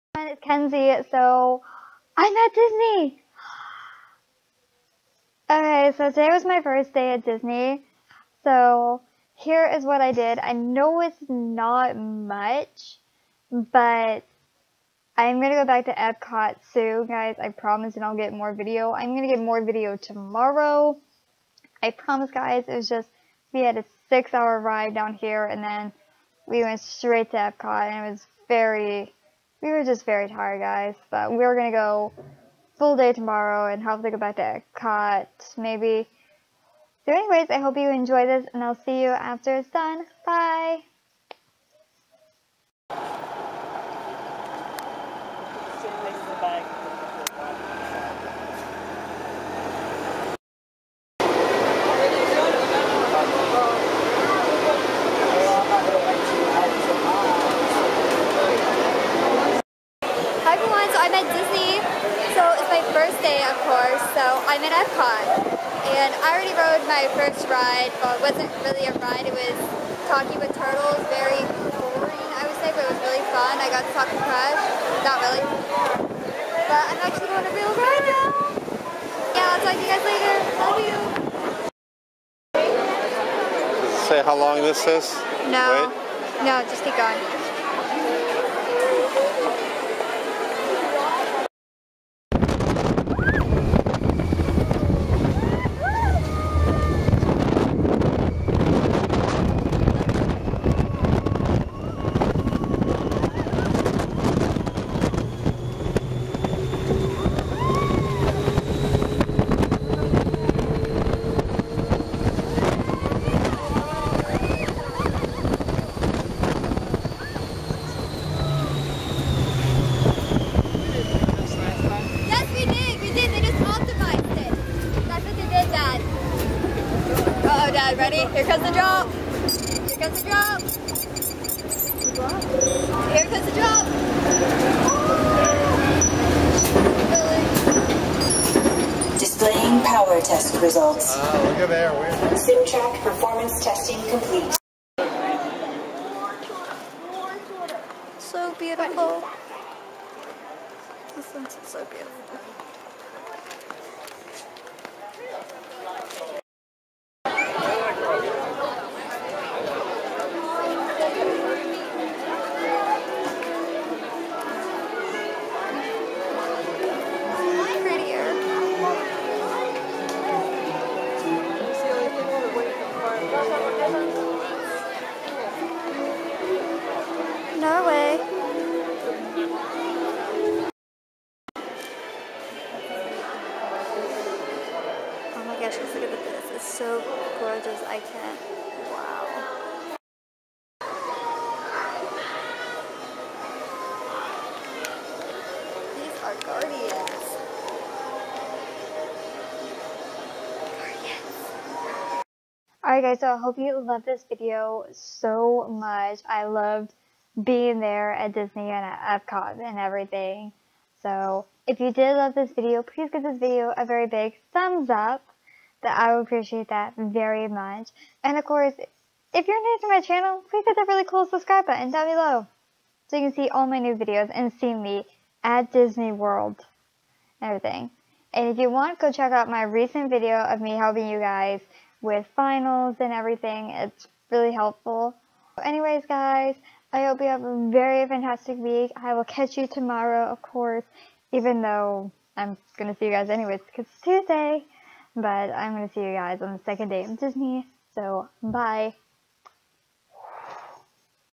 Disney World Day #1
Went to Disney World and went to Epcot. This is what I saw before it got real dark so I hope you guys like it.